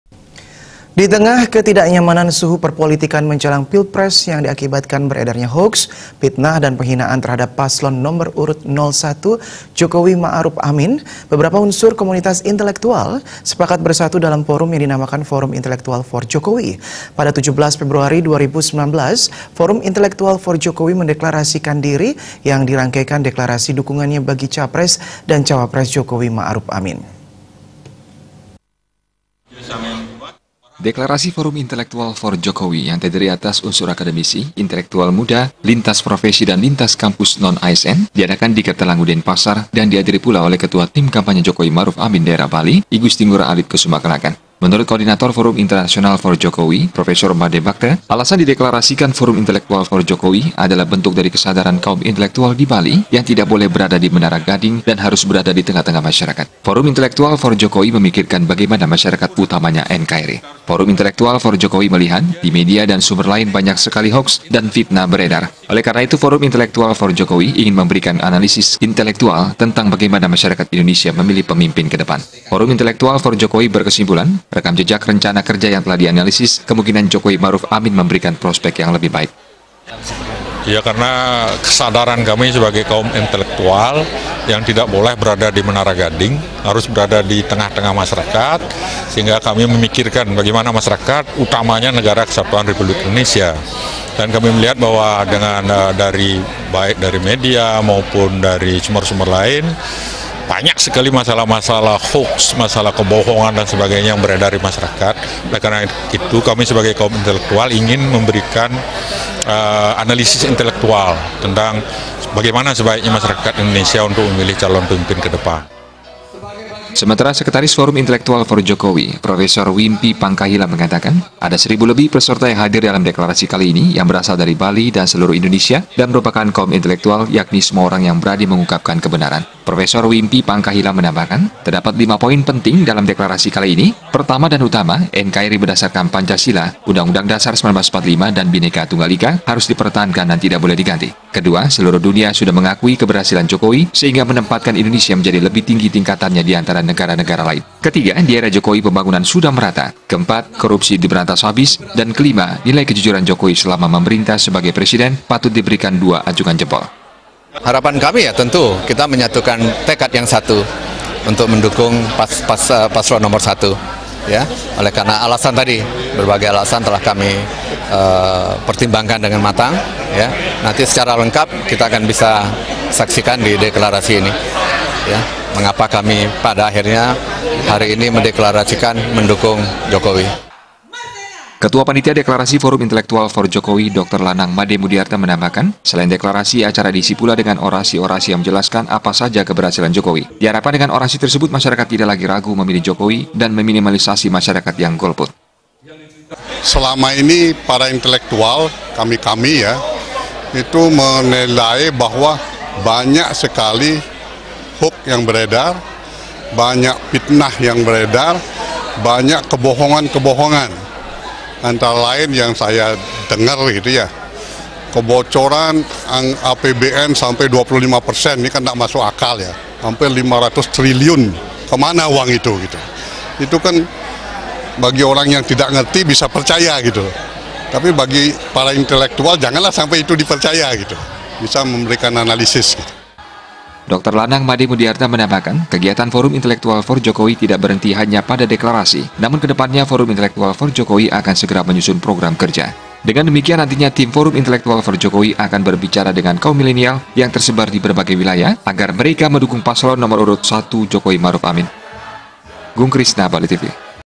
DEKLARASI FORUM INTELEKTUAL FOR JOKOWI
DITENGAH KETIDAKNYAMANAN SUHU PERPOLITIKAN MENJELANG PILPRES YANG DIAKIBATKAN BEREDARNYA HOAKS, FITNAH, DAN PENGHINAAN TERHADAP PASLON NOMOR URUT NOL SATU JOKOWI-MA’ARUF AMIN, BEBERAPA UNSUR KOMUNITAS INTELEKTUAL, SEPAKAT BERSATU DALAM FORUM YANG DINAMAKAN FORUM INTELEKTUAL FOR JOKOWI. PADA 17 FEBRUARI 2019, FORUM INTELEKTUAL FOR JOKOWI MENDEKLARASIKAN DIRI, YANG DIRANGKAIKAN DEKLARASI DUKUNGANNYA BAGI CAPRES DAN CAWAPRES, JOKOWI- MA’RUF AMIN.